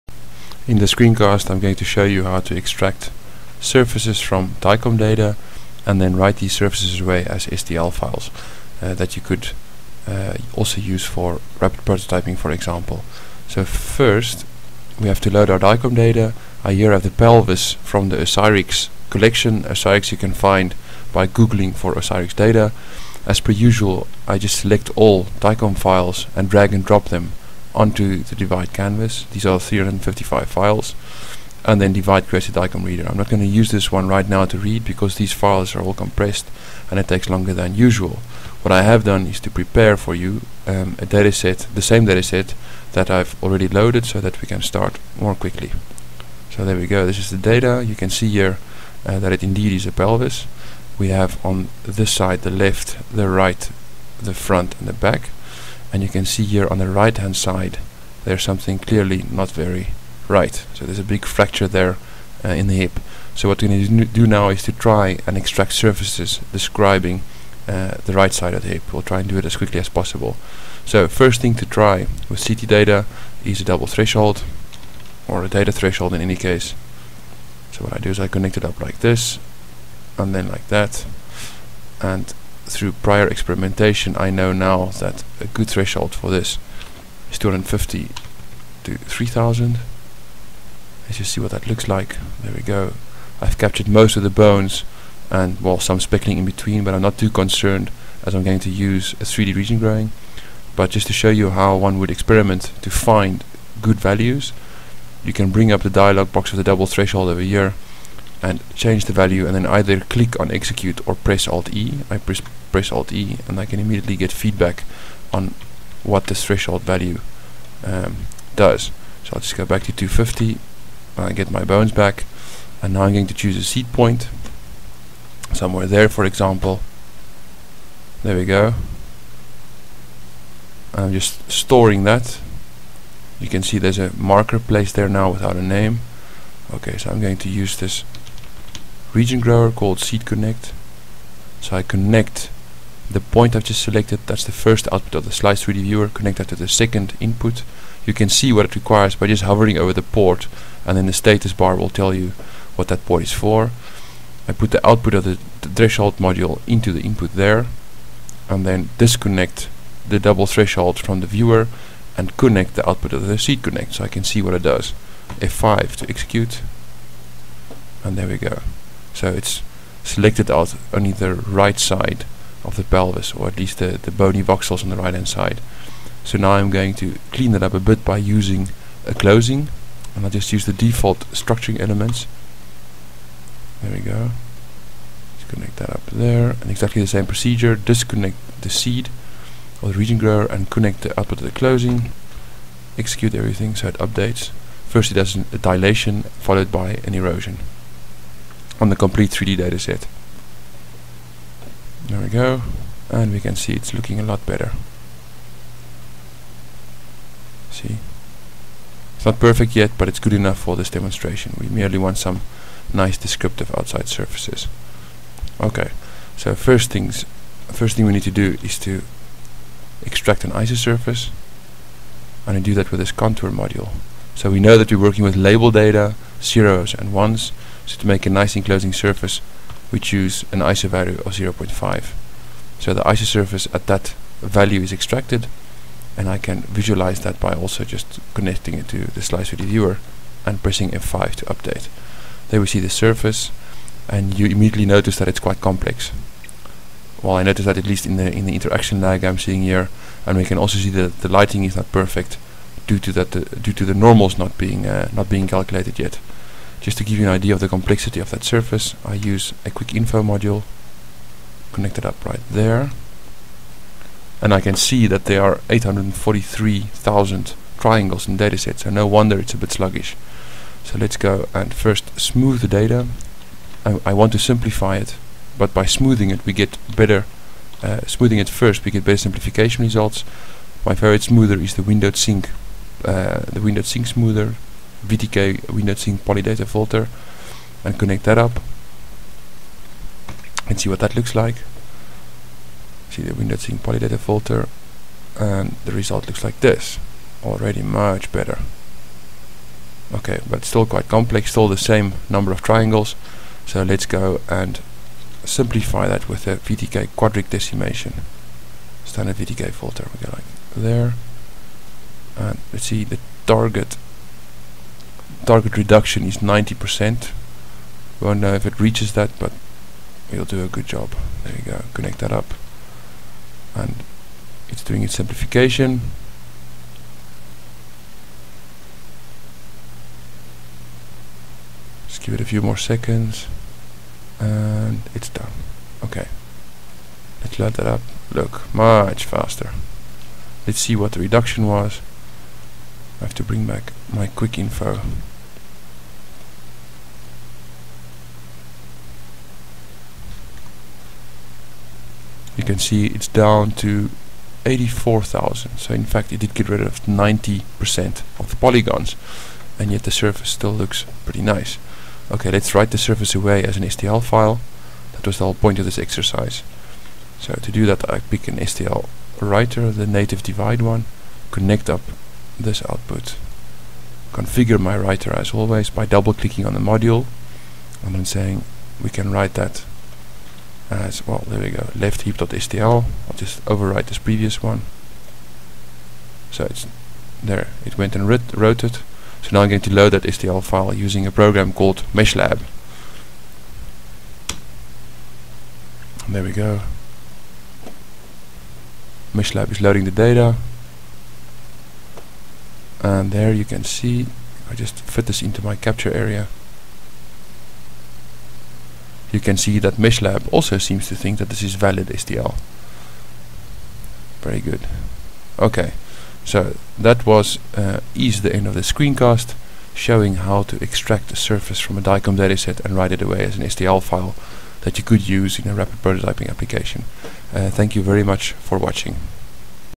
Extracting STL surface from CT data with DeVIDE
This screencast shows how to extract an STL format mesh surface from DICOM CT data, using the open source DeVIDE software [1].  Although DeVIDE can visualise the surface, the saved STL file is opened with another very cool open source package called MeshLab [2] to double-check its correctness.

* The final pipeline used in DeVIDE was:
DICOMReader - doubleThreshold - seedConnect - closing - contour - wsMeshSmooth - vtkQuadricDecimation - stlWRT
* I could have gotten rid of the inner surfaces by using a polyDataConnect with a seed point to select only the outer surface.